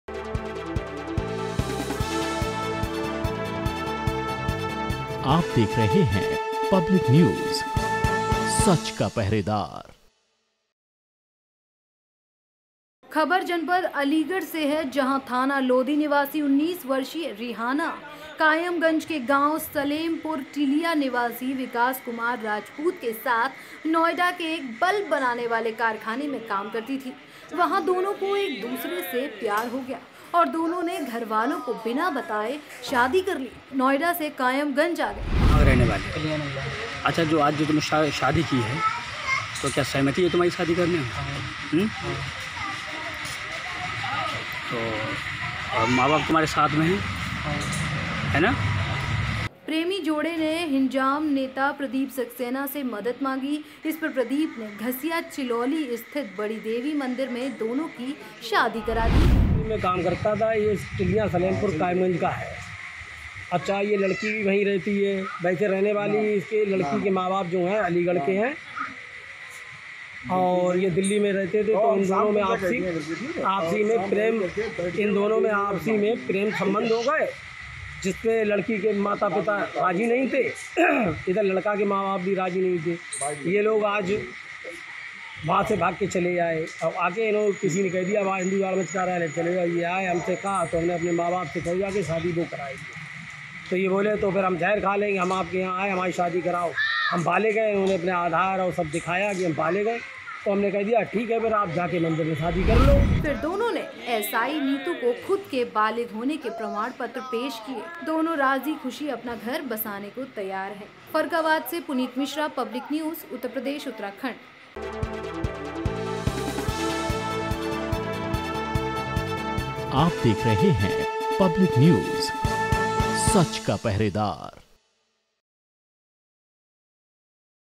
फर्रुखाबाद: मंदिर में शादी कर पुलिस की शरण में पहुंचा जोड़ा
नोएडा की एक फैक्टरी में साथ-साथ काम करने वाले प्रेमी जोड़े ने फर्रुखाबाद के कायमगंज के एक मंदिर में शादी रचा ली जिसके बाद दोनों सुरक्षा की गुहार लगाते हुए पुलिस के पास पहुंचे।

खबर जनपद अलीगढ़ से हैं जहाँ थाना लोधा निवासी 19 वर्षीय रिहाना कायमगंज के गांव सलेमपुर टिलियां निवासी विकास कुमार राजपूत के साथ नोएडा के एक बल्ब बनाने वाले कारखाने में काम करती थी। वहां दोनों को एक दूसरे से प्यार हो गया और दोनों ने घरवालों को बिना बताये शादी कर ली नोएडा से कायमगंज आ गए.

प्रेमी जोड़े ने हिंजाम नेता प्रदीप सक्सेना से मदद मांगी। इस पर प्रदीप ने घसिया चिलौली स्थित बड़ी देवी मंदिर में दोनों की शादी करा दी।  फिर दोनों ने  एसआई नीतू को खुद के बालिग होने के प्रमाण पत्र पेश किया।
इसके बाद एसआई नीतू ने बताया कि लोधा थाने की पुलिस के माध्यम से युवती के परिजनों से बात की गई तो पिता ने बताया कि जब वह घर से चली गई है तो उन्हें कोई एतराज नहीं है। इस पर पुलिस ने युवती को युवक को सुपुर्द कर दिया है दोनों राजी ख़ुशी अपना घर बसाने को तैयार हैं.